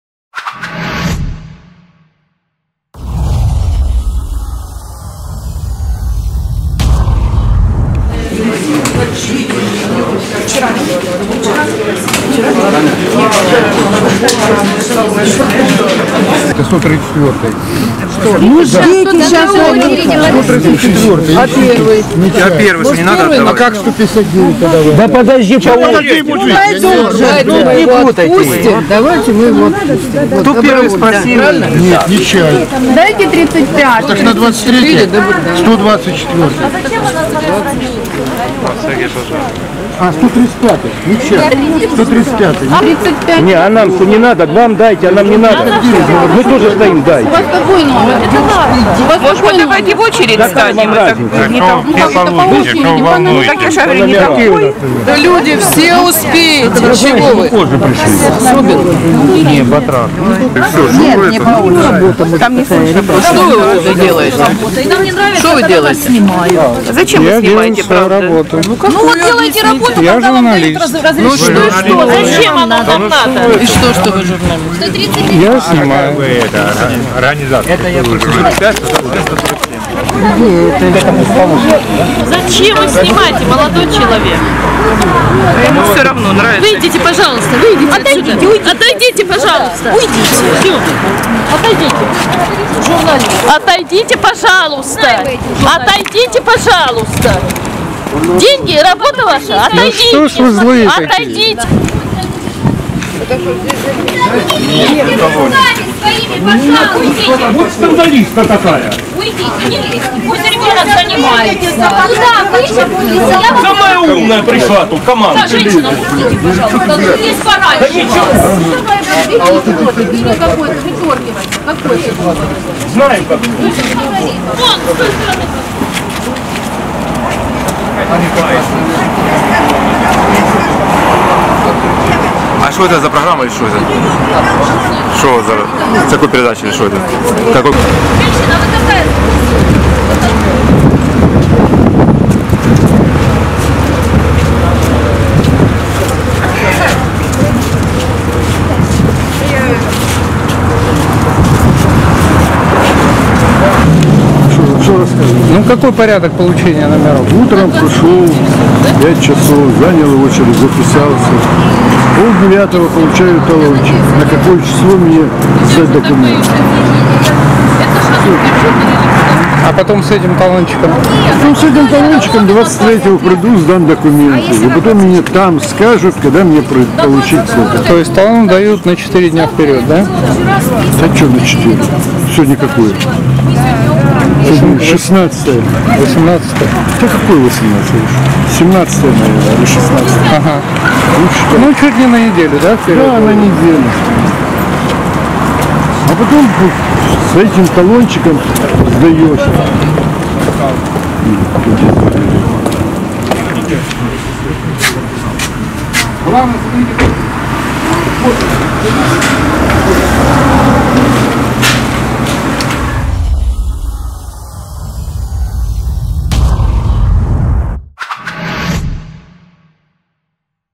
Запретный Донбасс. Как получить пропуск на оккупированные территории в Мариуполе
В Мариуполе 17 февраля в здании Жовтневой районной администрации продолжается прием документов для оформления пропусков в зону АТО. Получить их можно в несколько этапов- сначала надо записаться в очередь для получения талончиков. После получения оных назначат дату приема документов и через 10 дней, может быть, пропуск выдадут. Сегодня очередь писали на 24 февраля.